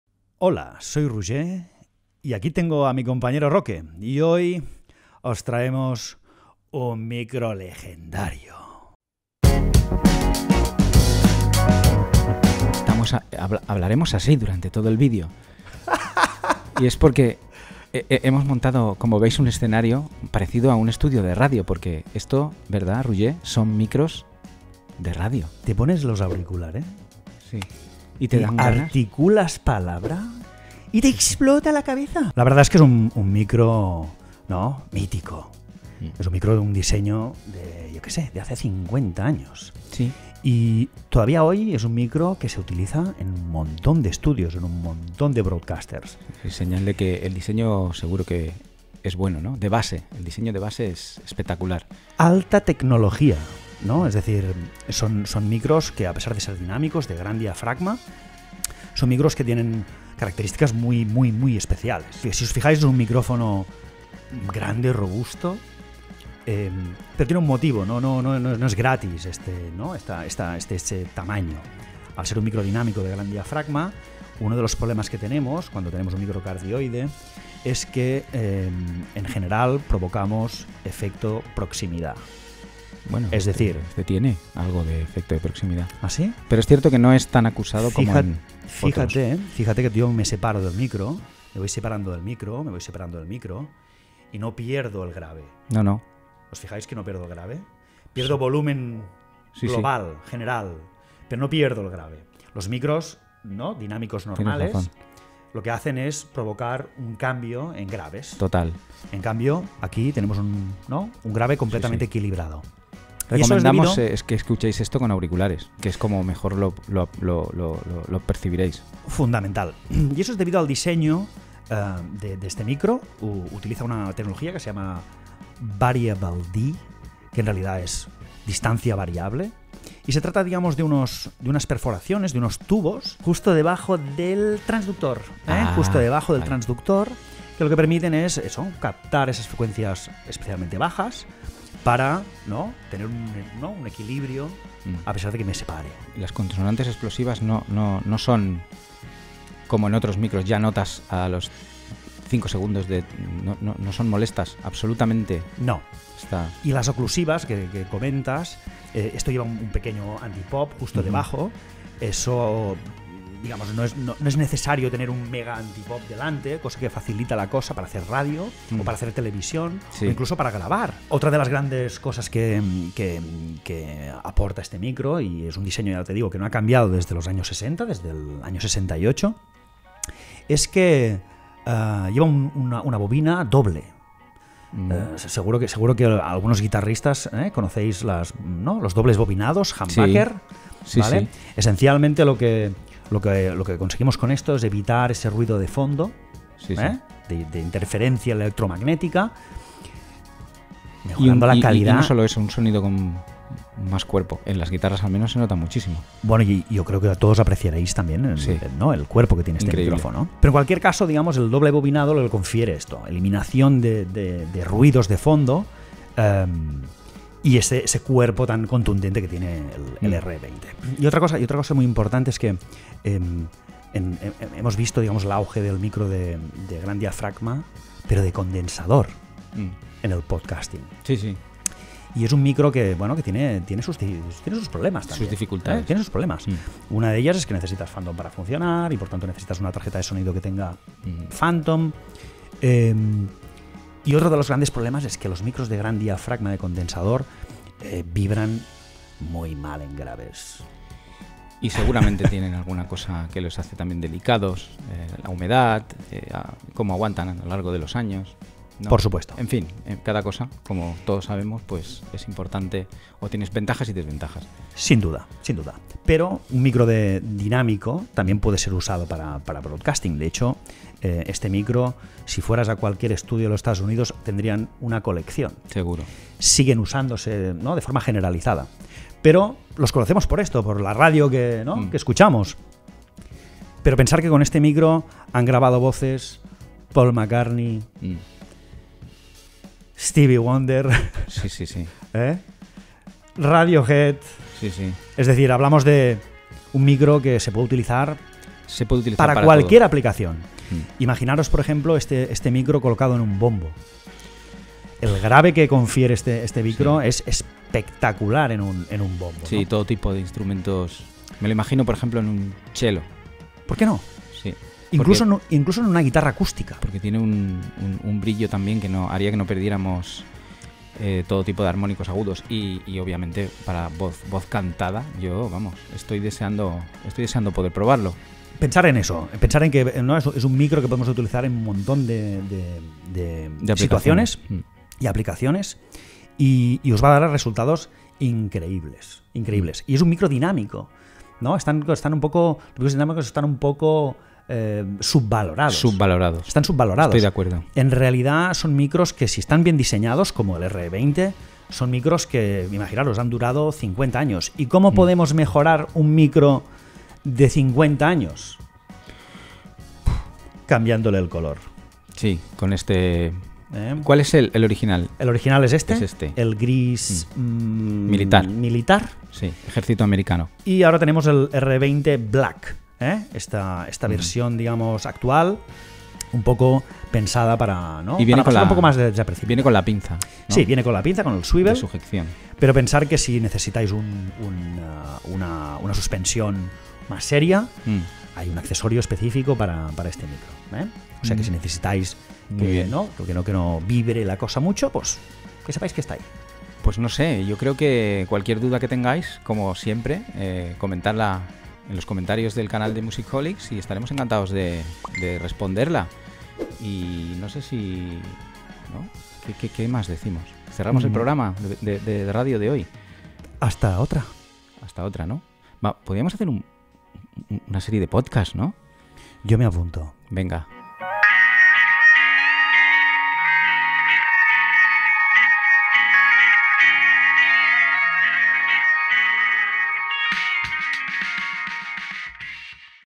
Electro Voice RE-20: micrófono dinámico de diafragma para Broadcast, Podcast, voces e instrumentos
RE20 es un micrófono dinámico de gran diafragma que se ha convertido en un estándar de la industria. Si bien tiene un uso muy extendido como micrófono de Broadcast y Podcast, su popularidad también se extiende a la producción musical, siendo muy utilizado como un micrófono para instrumento.

El diseño Variable-D™ y el filtro de pop interno de alta resistencia lo convierten en un micrófono ideal para voces.